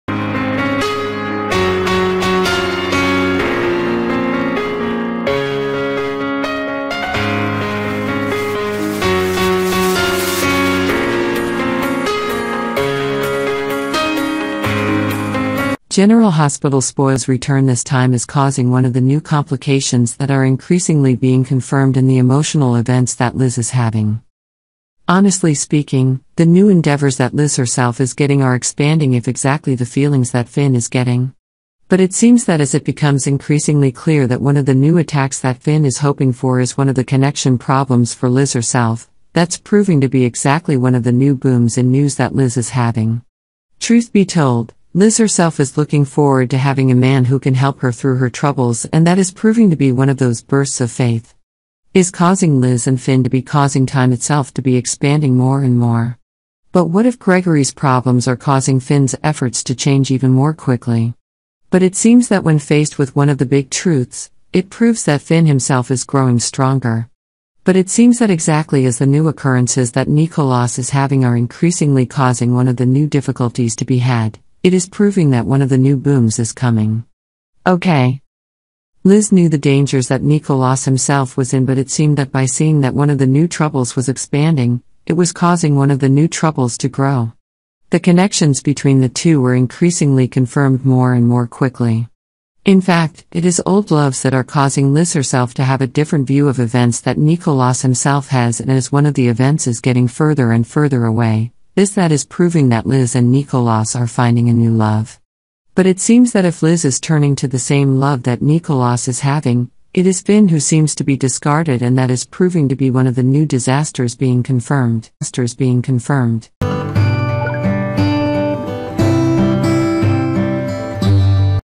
Liz chooses Nikolas, the shocking rejection of Finn's love General Hospital Spoilers Shocking
General Hospital Spoilers - General Hospital Podcast